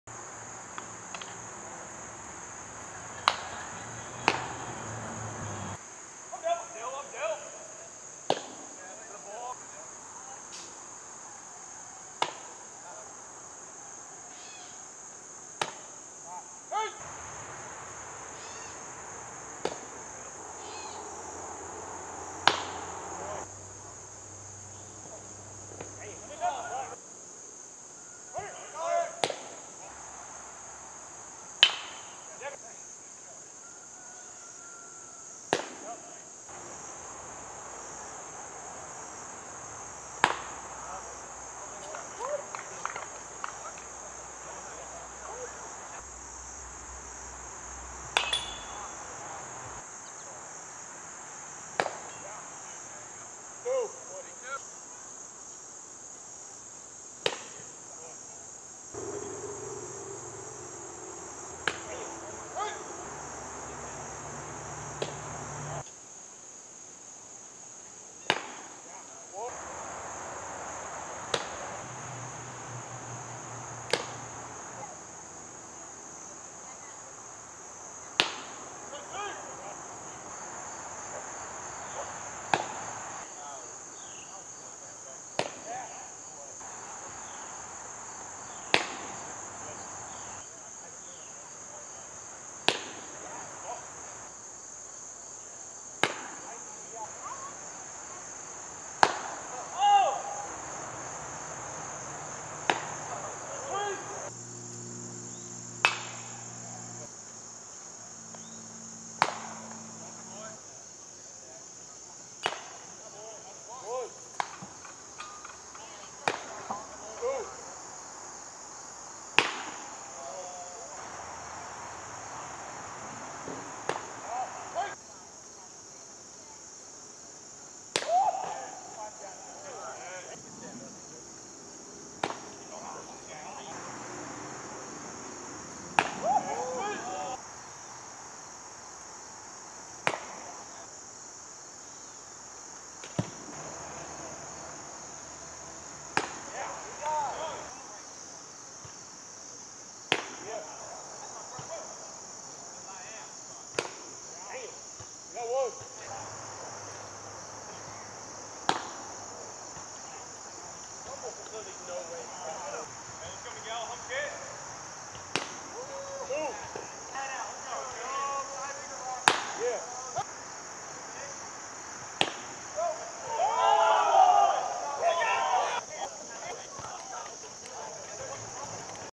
Ryan Stinar - 92-97mph T100 - World Series Highlights
Ryan Stinar - 28 year old Free Agent
Sat 95-97mph T100 first inning.
Settled in at 92-95 the rest of the game.
7ip 2 hits 12 ks 1ER

VF World Series 2022 Game 4 Highlights